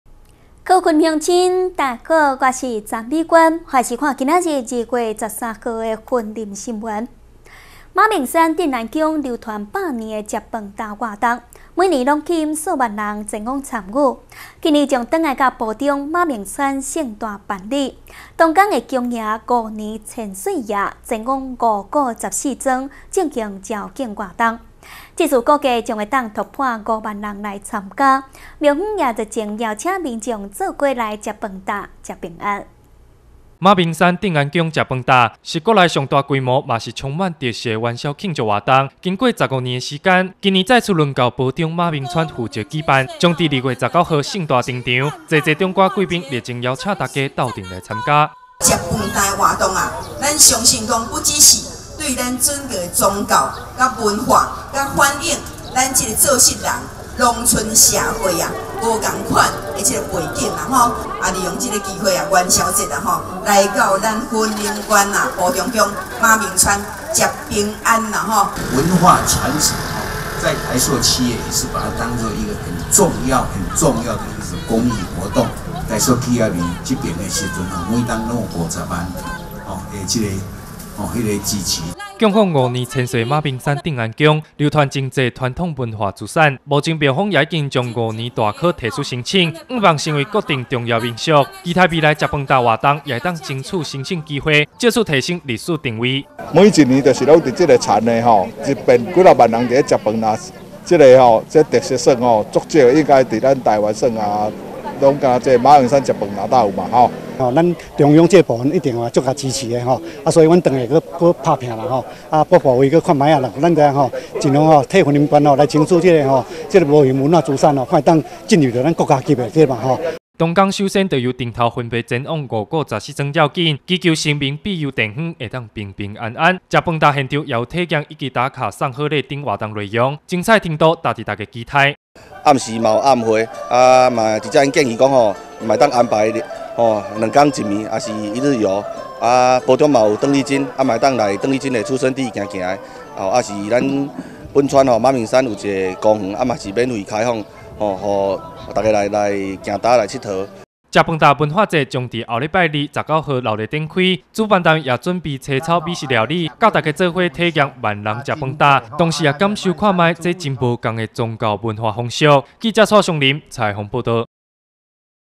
雲林新聞網-馬鳴山鎮安宮萬人吃飯擔 下周二盛大登場
萬人吃飯擔保平安，壯觀場景不容錯過~~